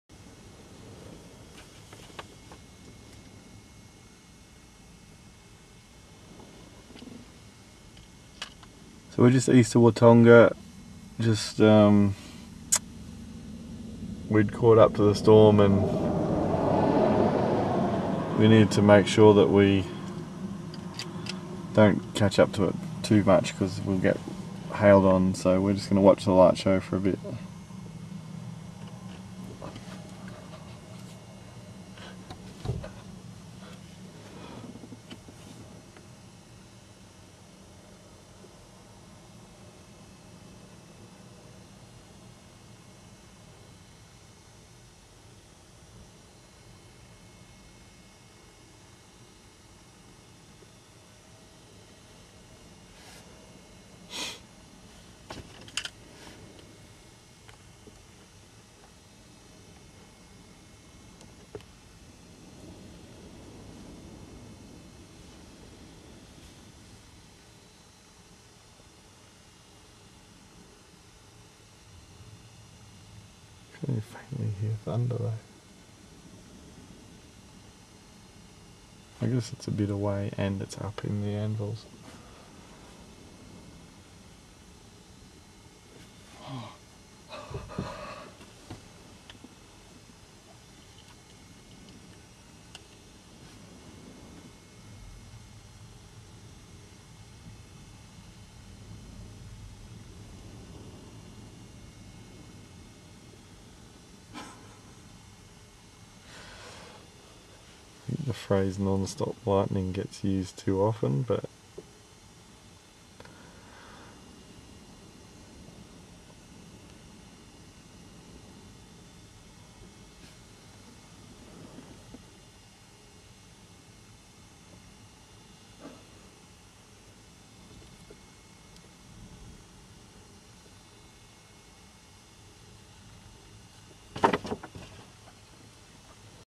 Oklahoma Light Show Part 1 May 23 2011
Evening of May 23 2011 , just east of Watonga looking eastward.